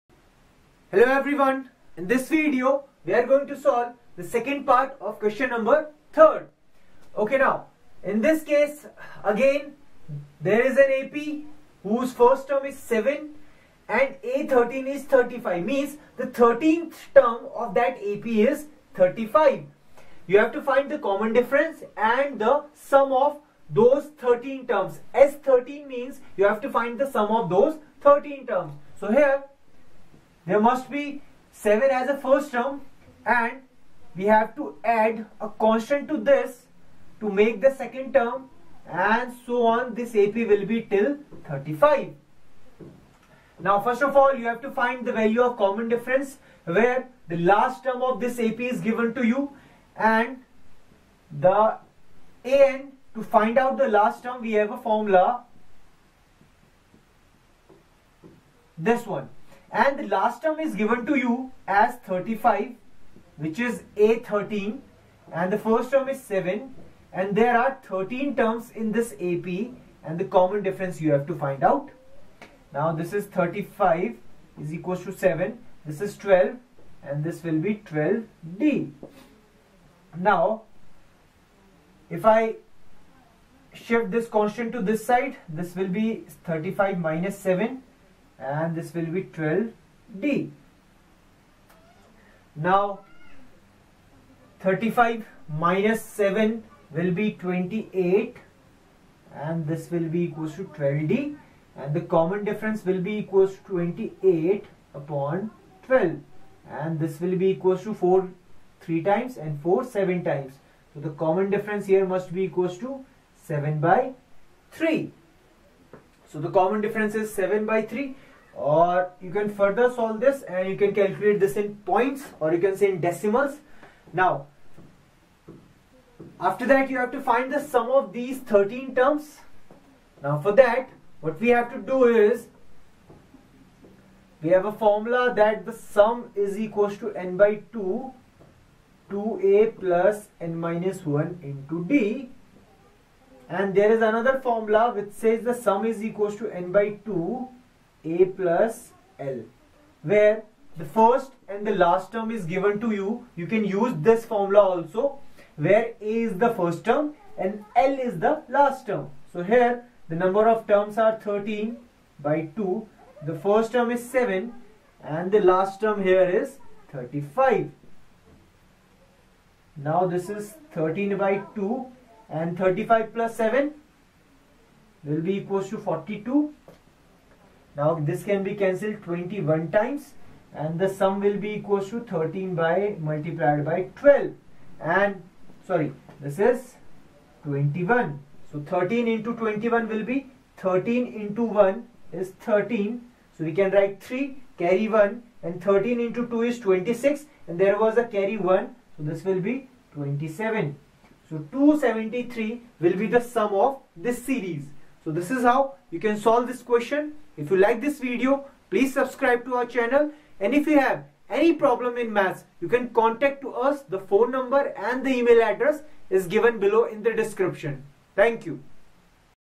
Exercise 5.3 - Question 3 (ii) NCERT/CBSE Solutions for Class 10th Maths Arithmetic Progressions
Exercise 5.3 - Question 3 (ii) NCERT/CBSE Solutions for Class 10th Maths Arithmetic Progressions || True Maths